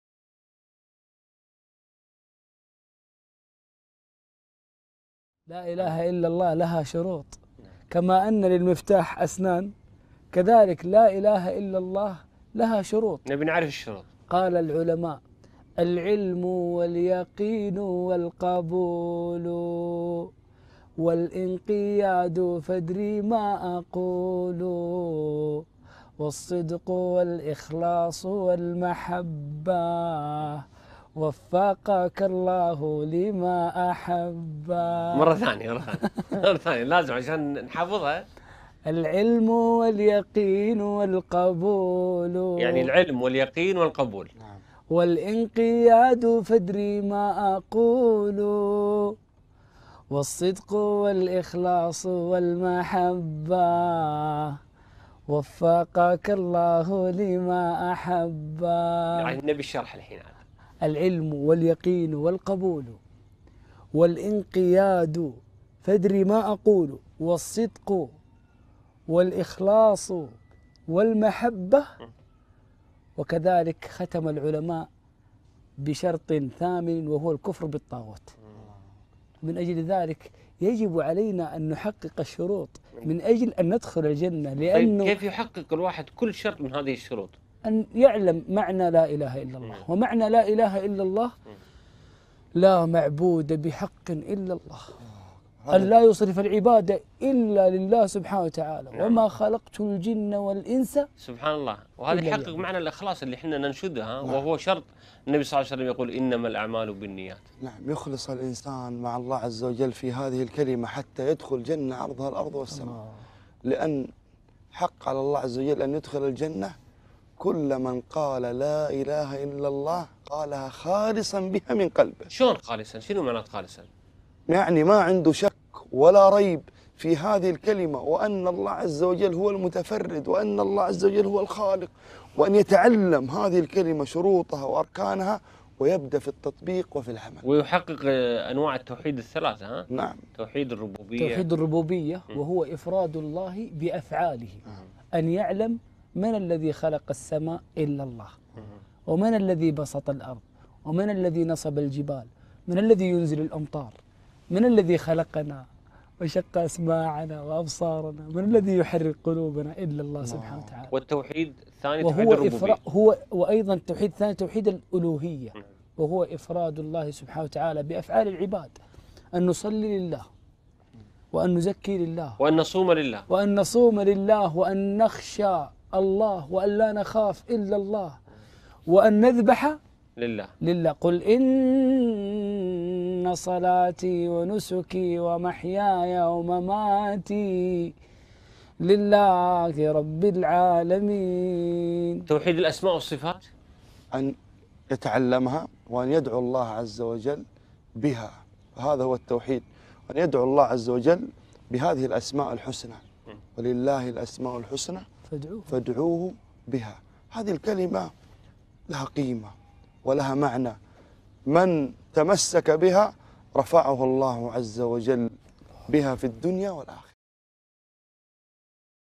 7 Conditions of 'La ilaha illa Allah’ - Mansur Al-Salimi - الشيخ منصور السالمي
The conditions of la ilaha illa Allah are seven, as in the poem of the scholar Hafidh al-Hakami.

Here, they are summarized by Sheikh Mansur Salimi and Nayef Sahafi, as well as a summary of the three types of tawheed.

Conditions of La ilaha illa Allah, Seven:
1.) Knowledge
2.) Certainty
3.) Acceptance
4.) Submission
Know, what I am saying.
5.) Truthfulness
6.) Sincerity
7.) And love
May Allah allow us to obtain what he loves.

شروط لا إله إلا الله.  ٦

العلم
واليقين
والقبول
والانقياد
فادر ما اقول
والصدق
والإخلاص
والمحبة

 وفقك الله لما احبه